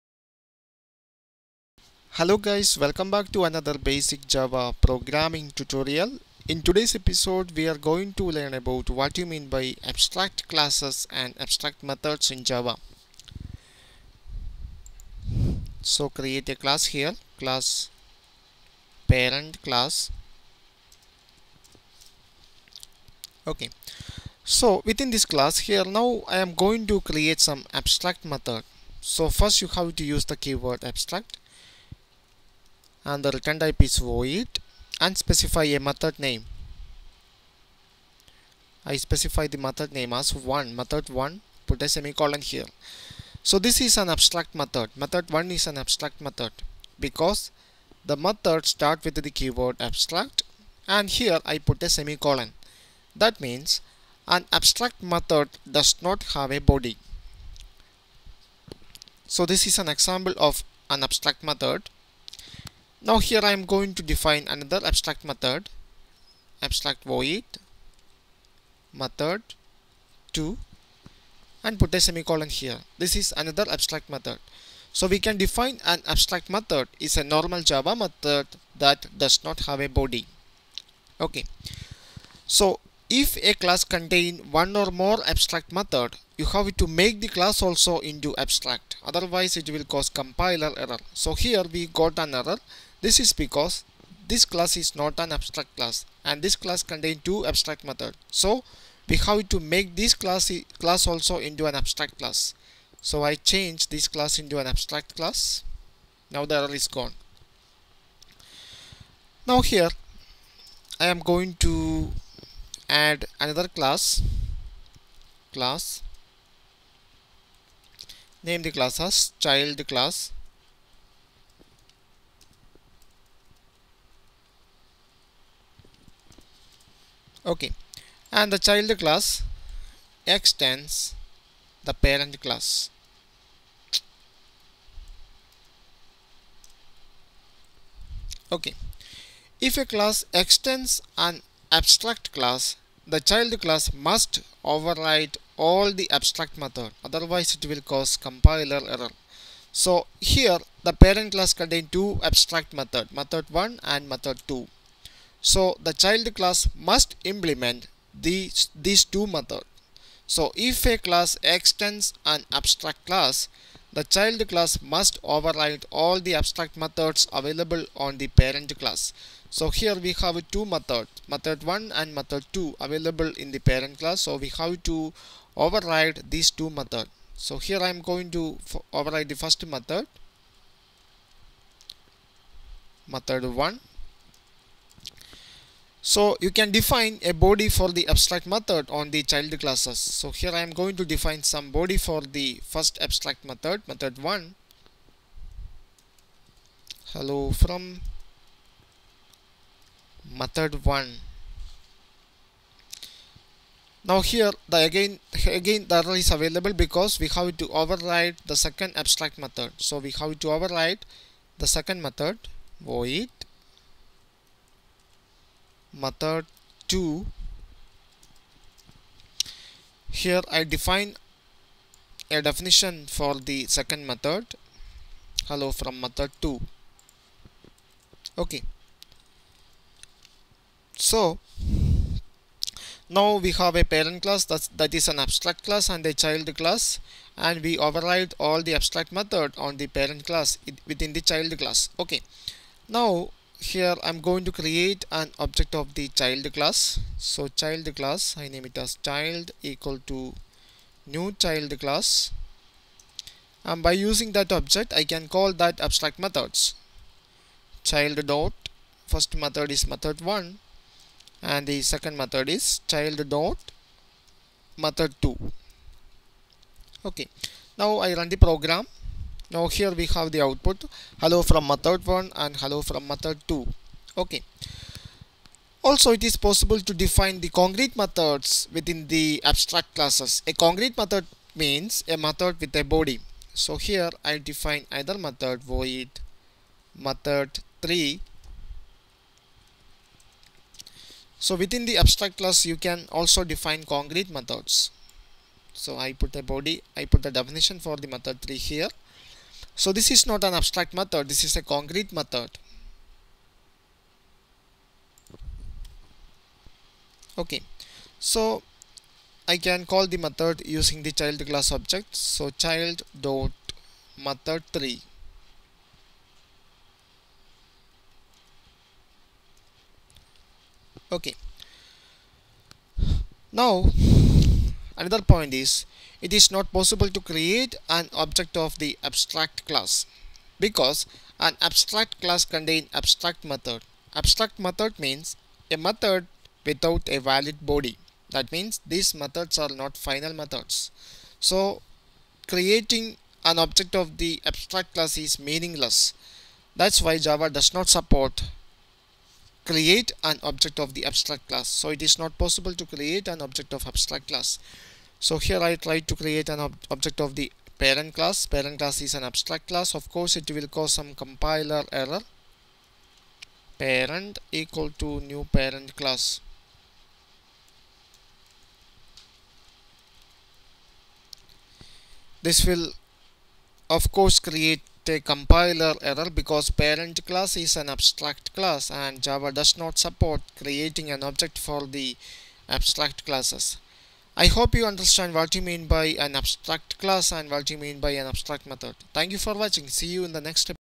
Core Java - 27 - Abstract Classes & Methods.
This video demonstrate what is the use of abstract classes & methods in Java.